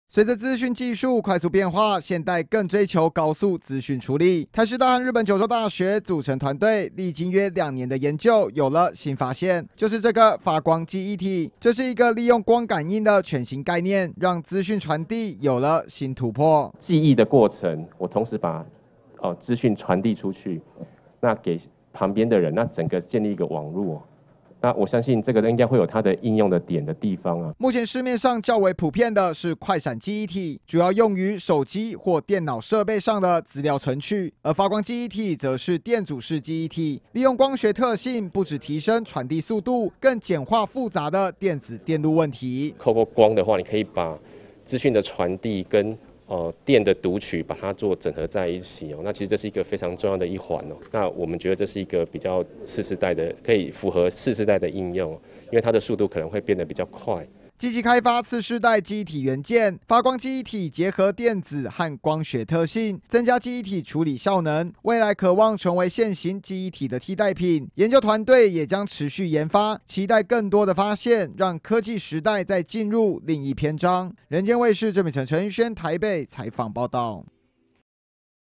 20210923 台日聯手！"發光記憶體"加速運算效能
科技再進化。由台師大光電工程研究所和日北九州大學，共同組成的研究團隊，利用光學特性，發現另一種記憶體模式；這項技術不只簡化原本複雜的電子電路問題，更可望實現快速運算需求，未來也期待開拓更多元的科技應用。